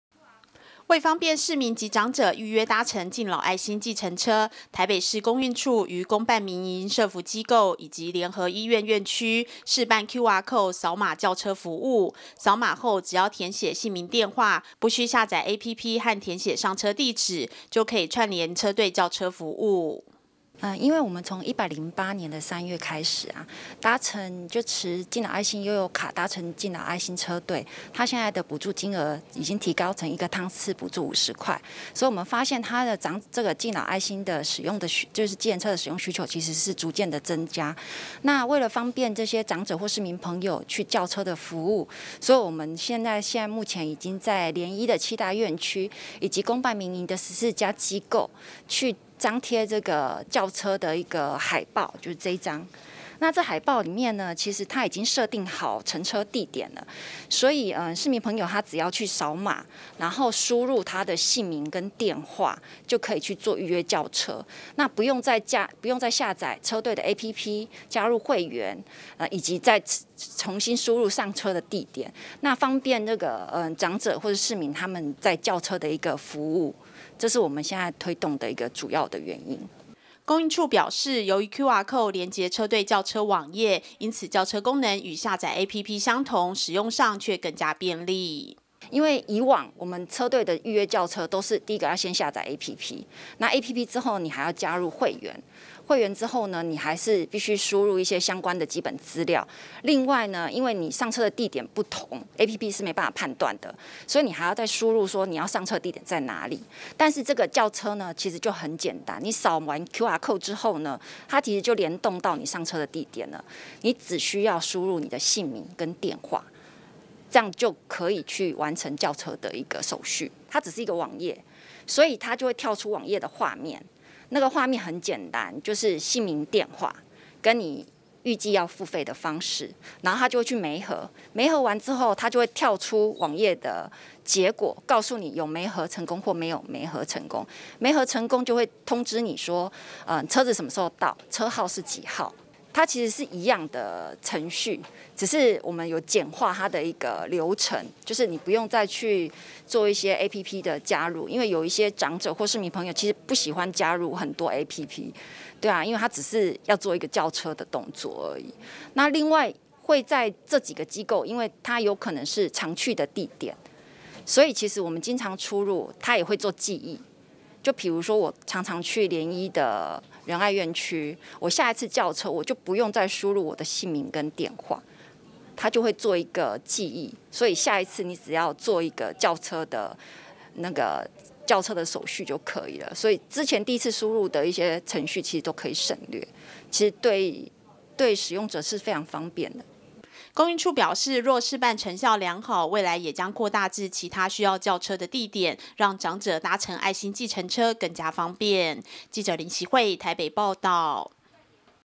愛心計程車|掃碼叫車|方便長者搭車 北市23處試辦掃碼叫車服務
為方便市民及長者預約搭乘敬老愛心計程車，台北市公運處於公辦民營社福機構以及聯醫院區，試辦QR code掃碼叫車服務，掃碼後只要填寫姓名電話，不須下載App和填寫上車地址，就可以串接車隊叫車服務，提升長者叫車的便利性。